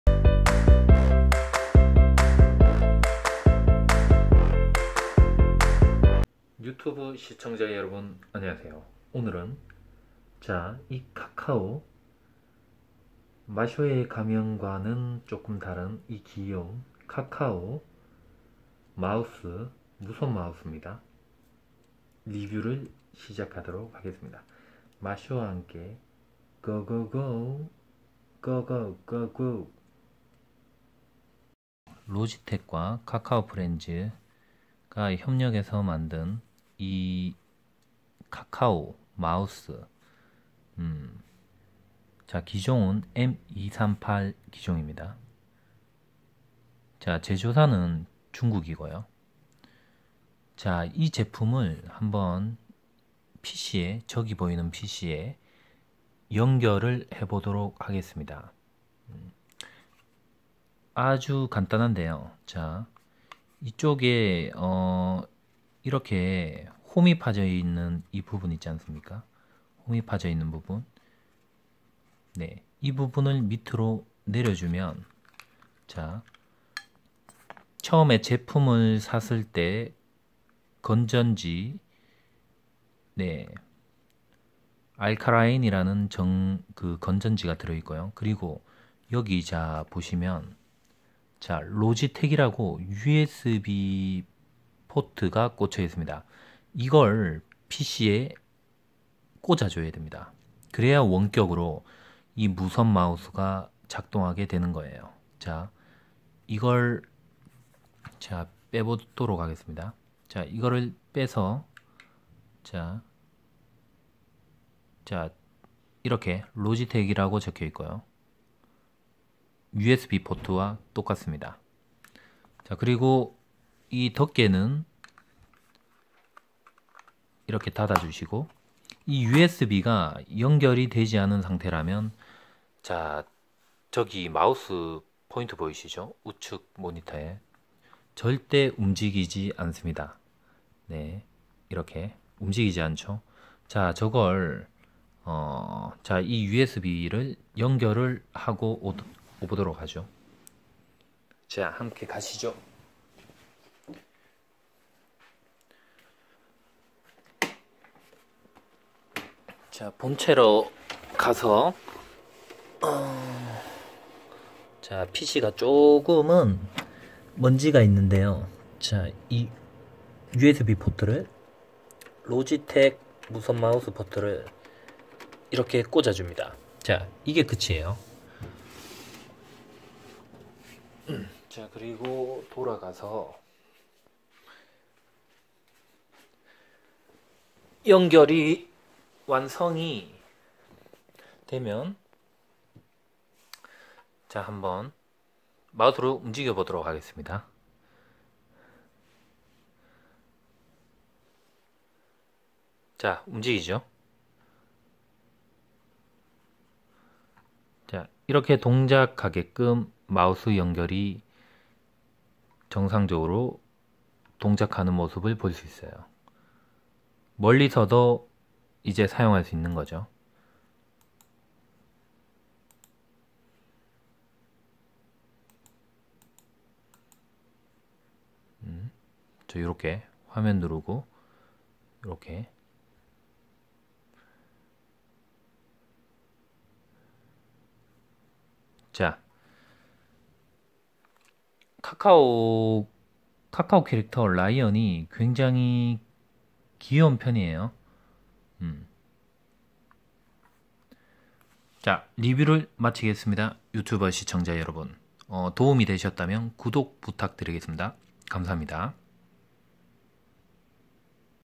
카카오무선마우스 제품소개 및 리뷰 (설치법 소개)
귀엽고 앙증맞은
마쇼의 ♥카카오무선마우스♥ 제품소개 및 리뷰^^